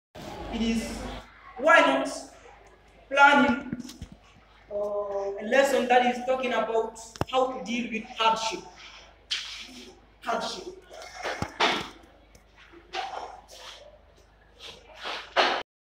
An introduction of a lecture
In my classroom in the University General Lansana of Sonfonia precisely at the English language department I found the necessity to plan a lesson which talks about:"How to deal with hardship in life" .
In fact my classmates and I we enjoyed the topic.